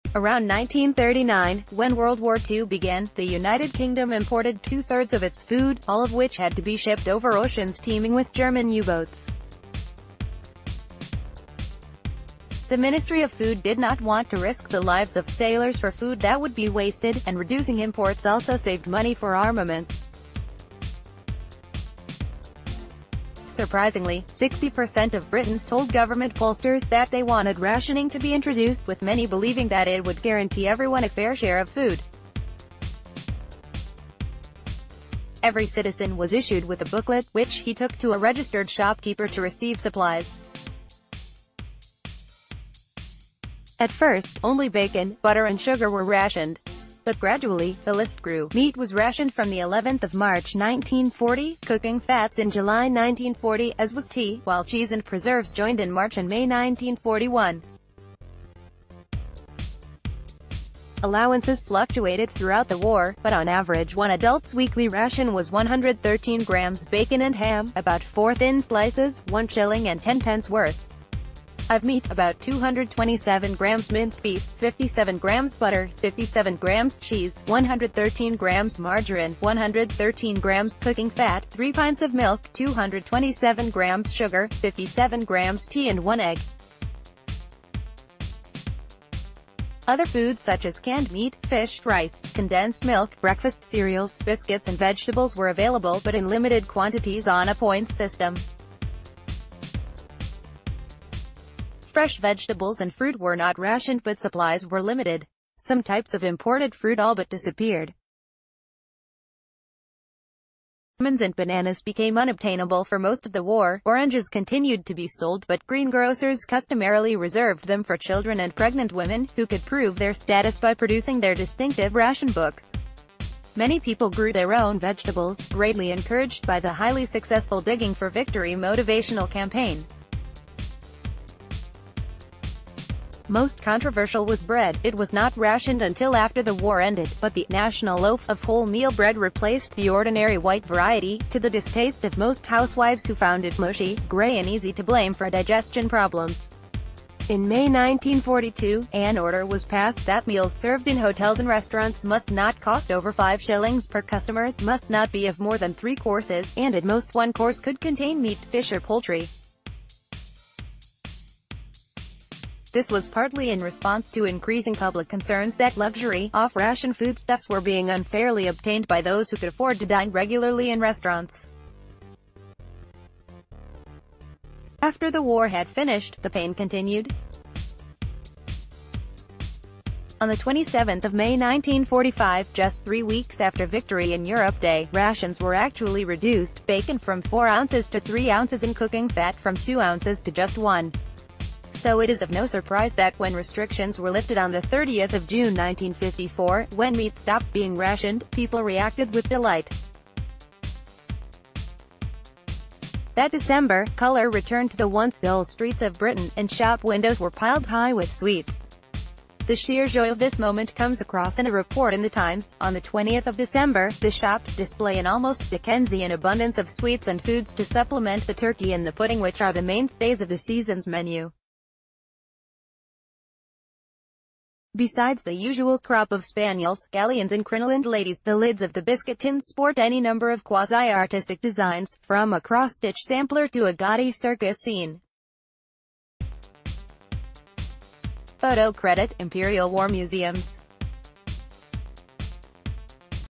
The weekly food ration for two people, UK, 1943
Around 1939, when World War II began, the United Kingdom imported two-thirds of its food, all of whi...